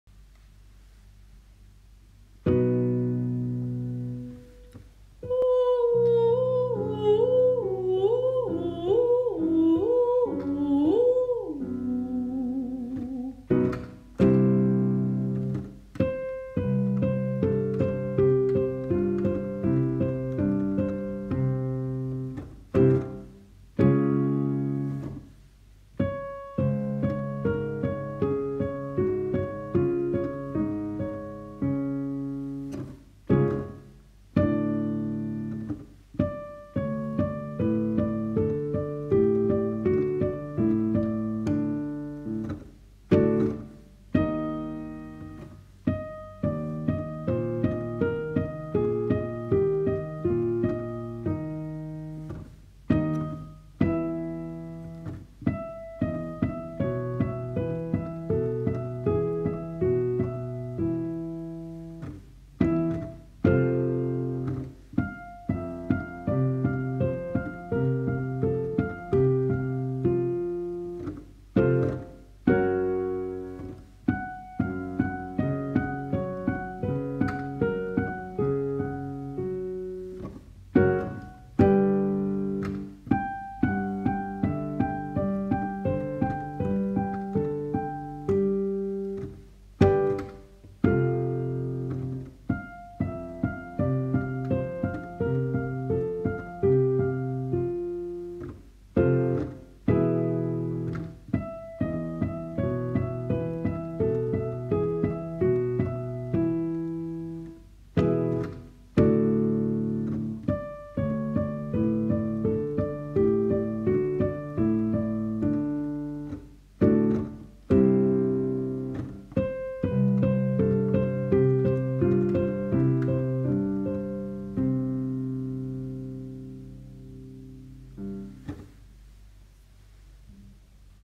ÜBUNG: Intervallen-Übung
Diese Übung hilft Ihnen die innere Räume für hohe Töne zu trainieren. Wenn Sie die Übung machen, stellen Sie sich vor, dass Sie sich vor dem Singen auf eine Überraschung freuen. Aber richtig freuen! Damit die innere Räume sich öffnen können.

Auch im Blog gibt es viele Artikeln über die Stimmentwicklung und erfolgreiches Präsentieren